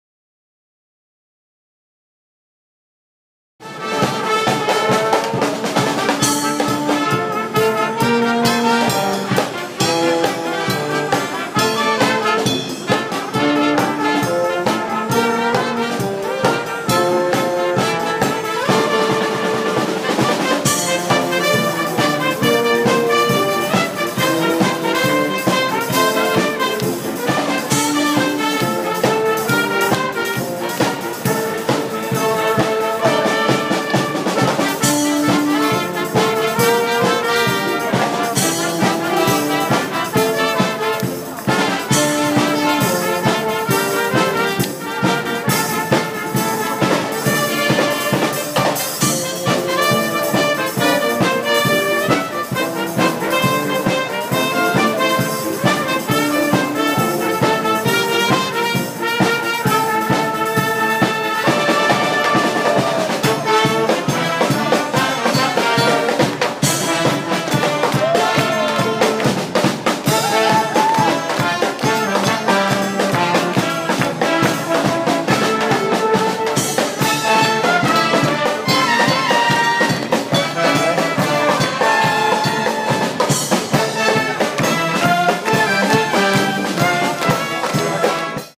Paris Street band
When we got to Paris on June 21, there was a city-wide music festival going on. Here's one group we encountered.